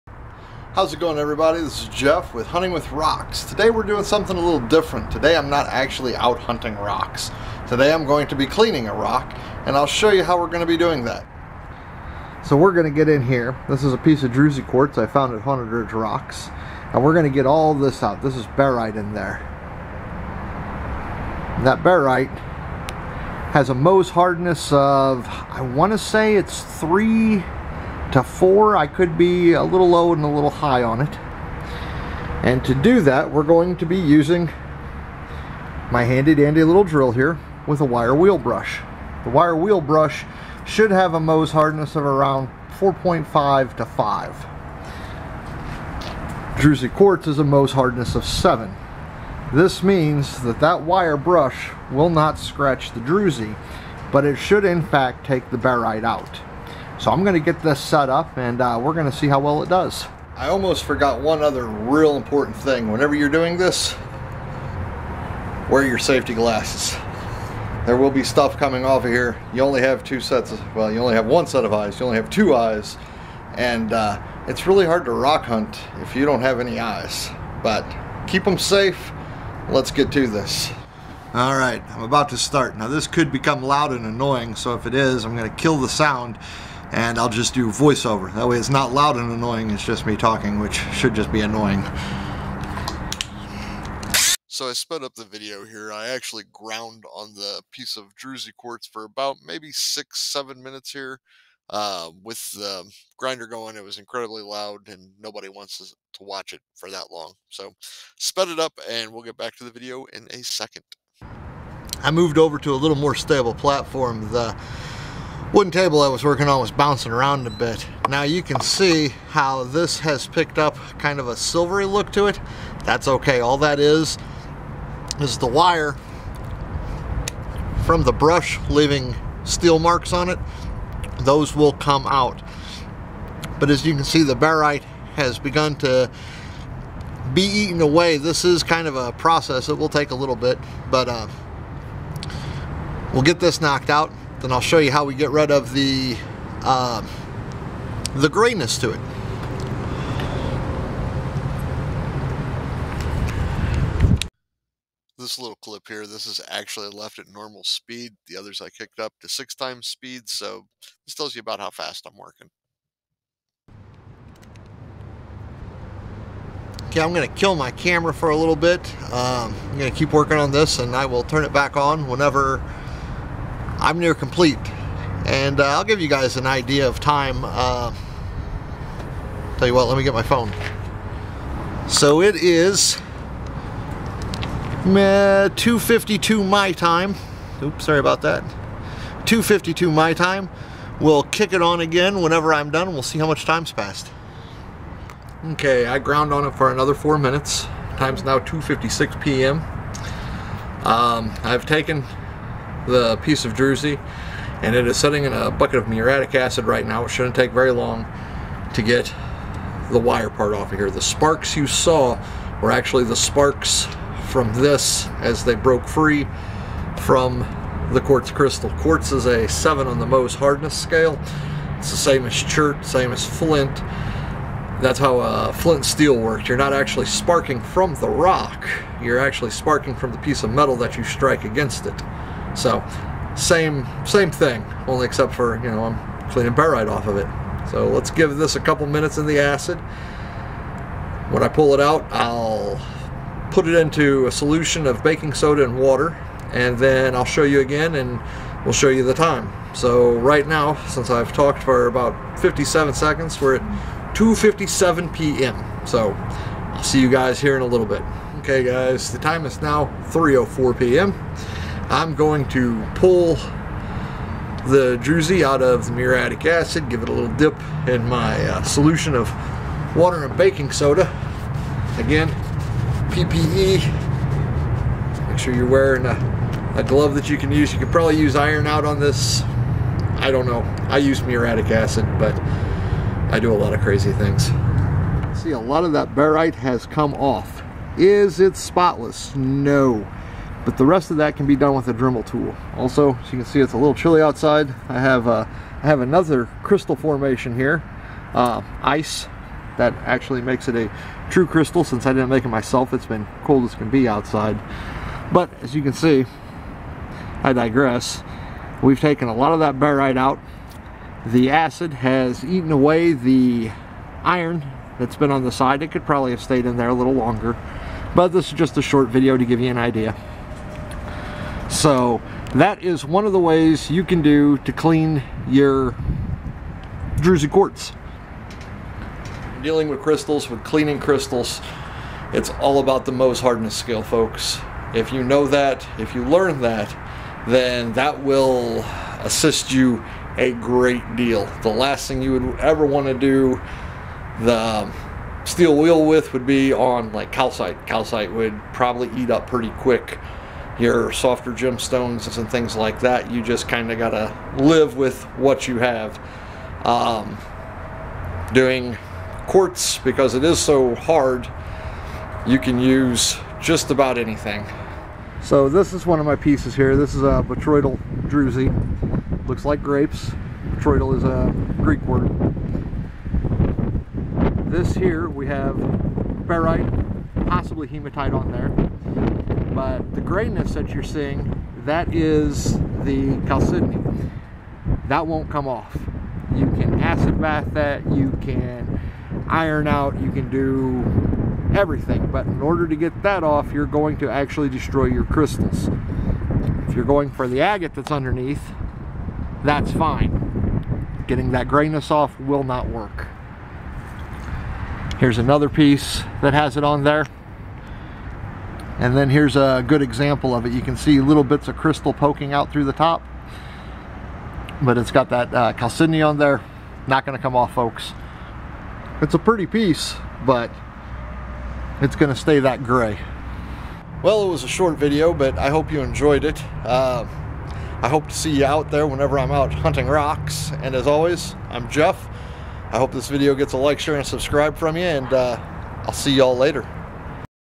Clean a crystal with a wire brush ?!?!?
So in this video I use my knowledge in the Mohs hardness scale, learning it will make your life so much easier when it comes to cleaning rocks .

Here is a link to a fairly large list ( but by no means complete) of minerals on the Mohs Hardness scale.